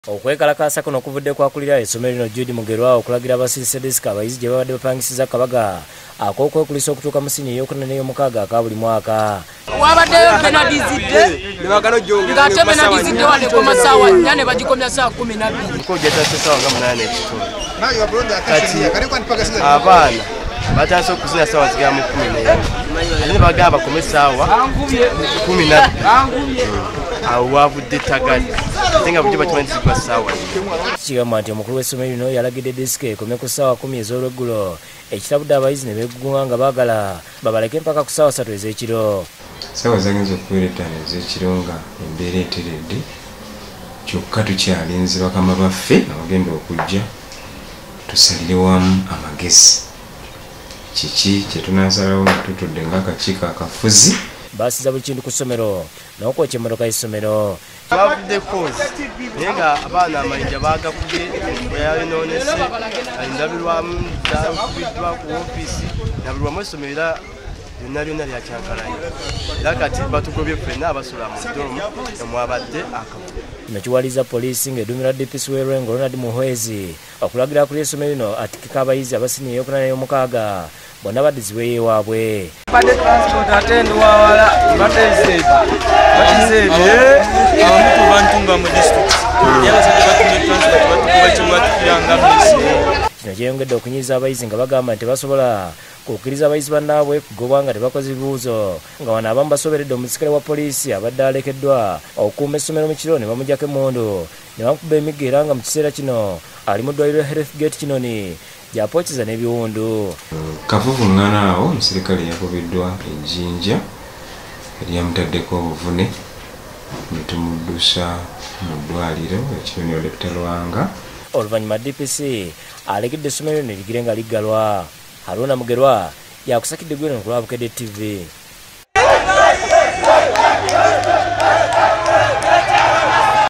Abayizi beekalakaasizza lwa ndongo
Bakubye owa Poliisi essomero neriggalwa.